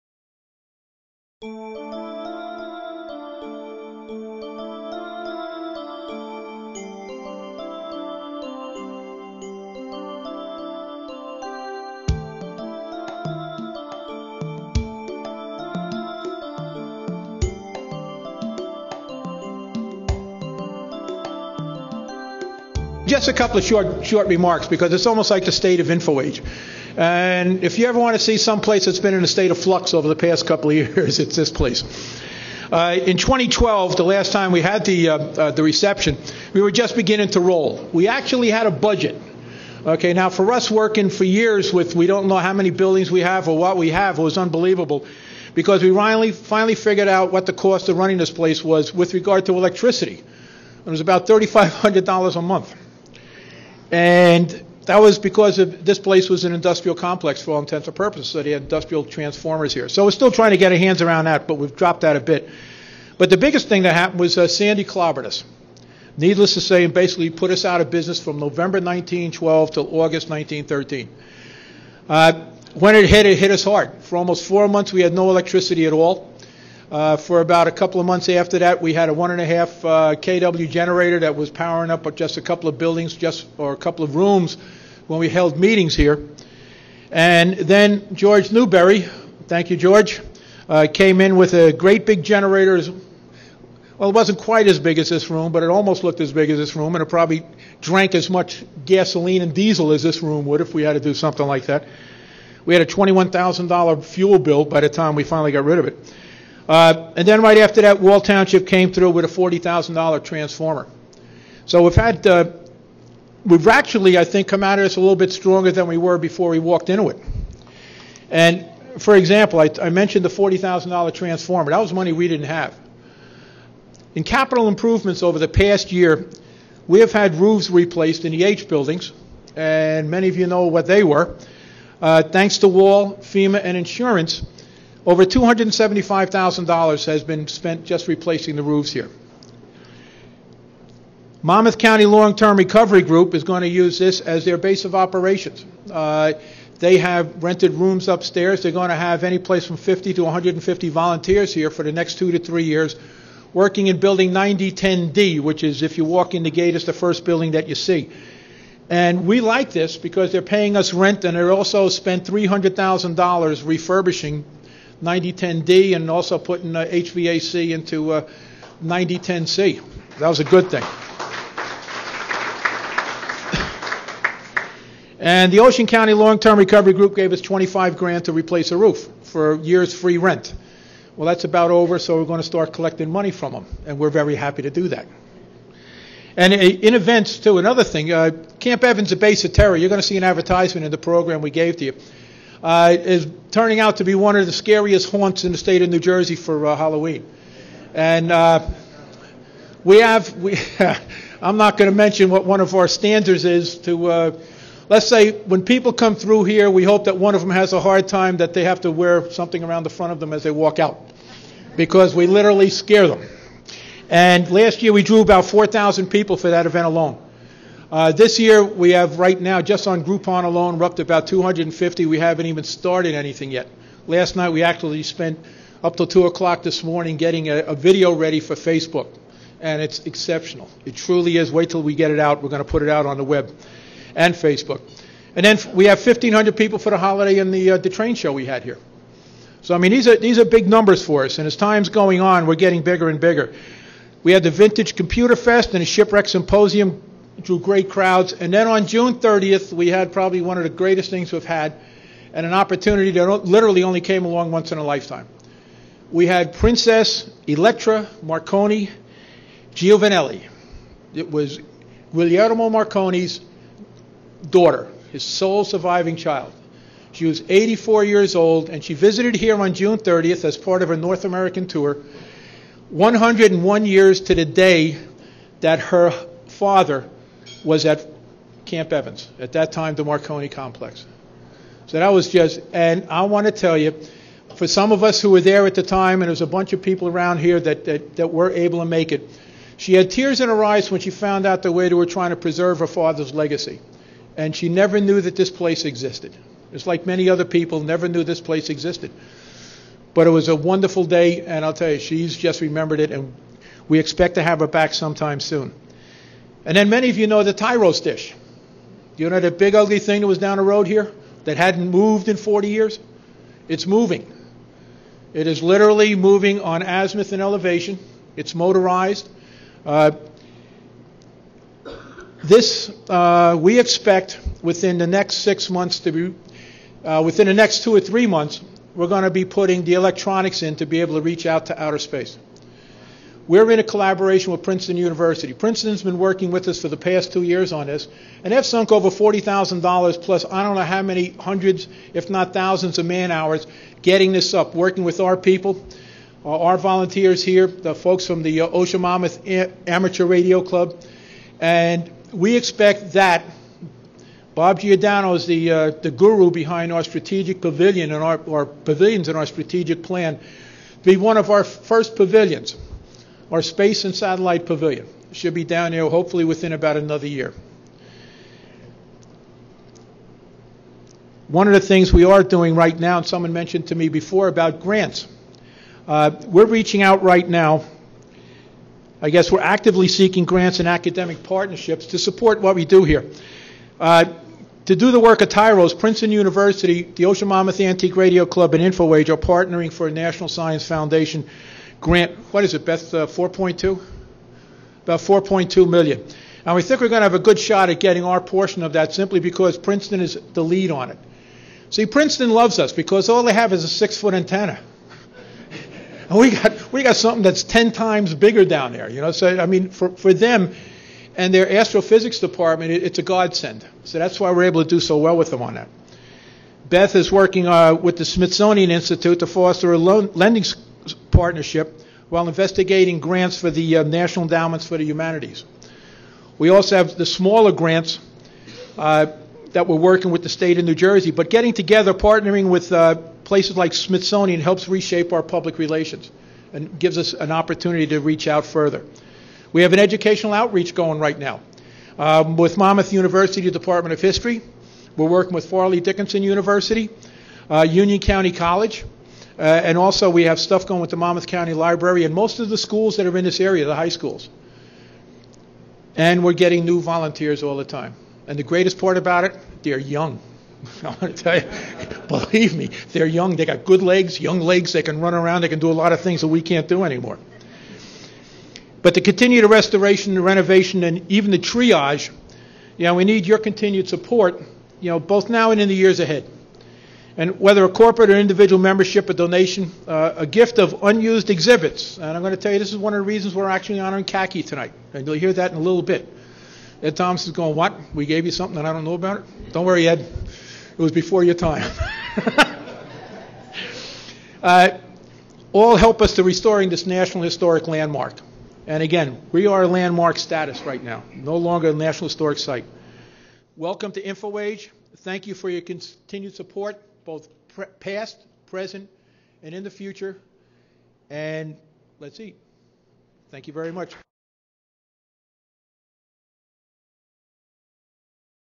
The State of InfoAge - September 2014
A brief recap of recent progress at InfoAge. Introductory remarks by Michael T. Ruane, Chairman of InfoAge at the Wall of Honor Ceremony, September 20, 2014.